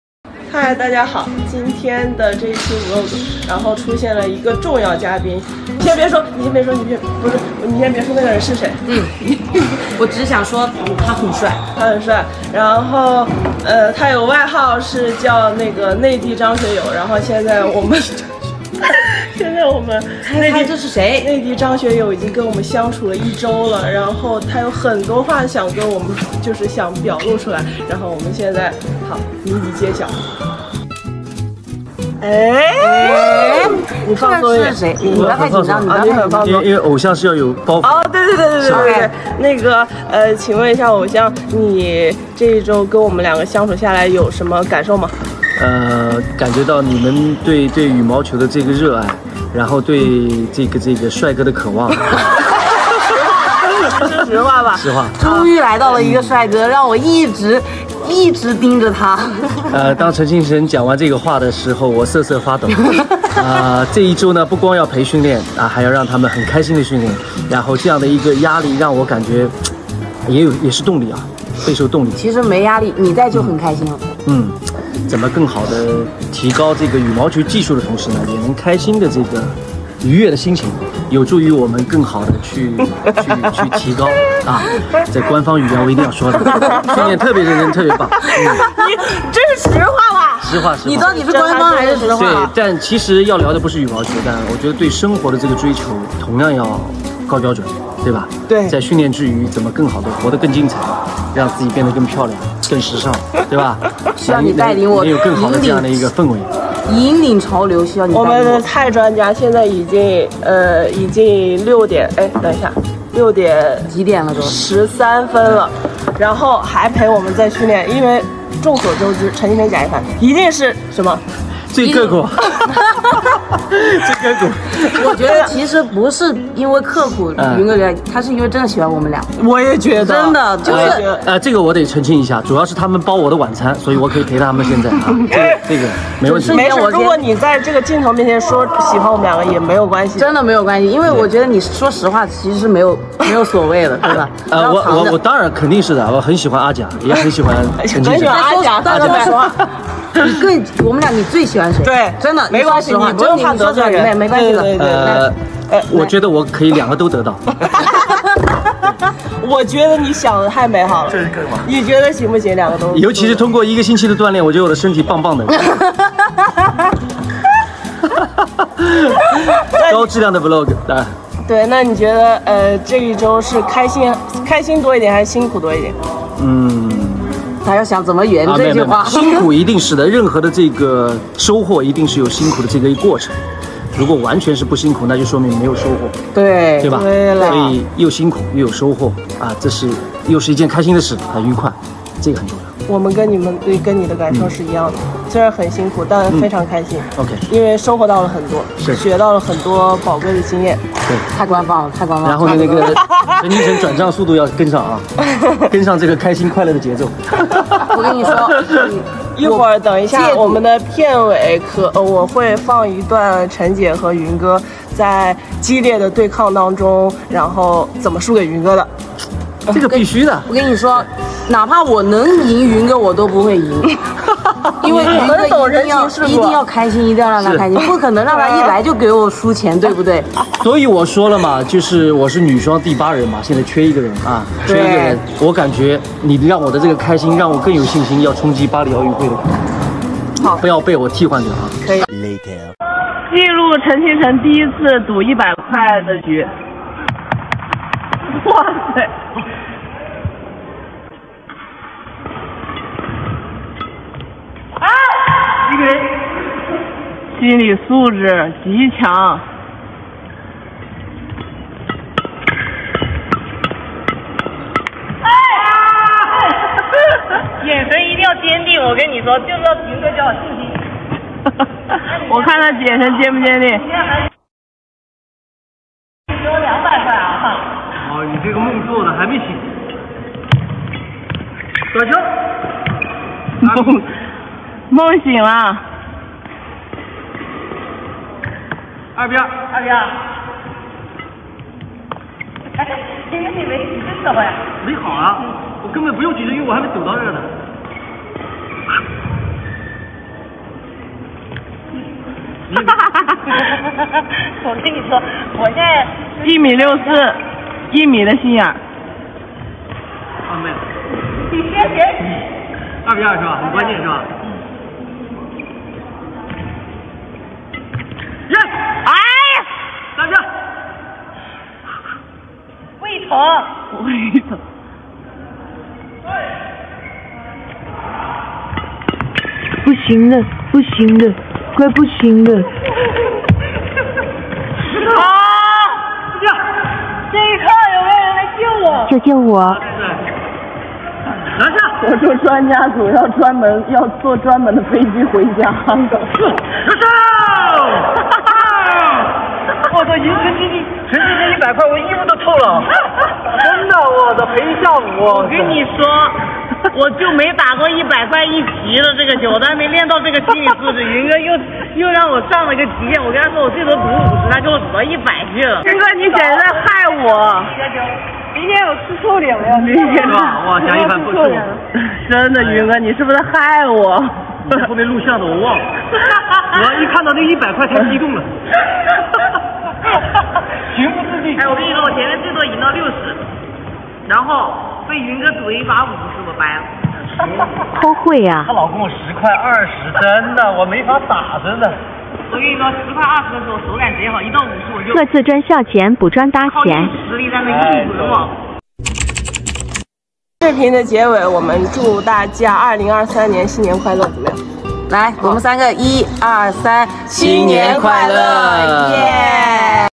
【贾一凡的vlog】跟蔡专家相处了一周，收获繁多
羽毛球,体育,世界冠军,运动,教练,国羽,陈清晨,贾一凡,羽毛球女双,凡尘组合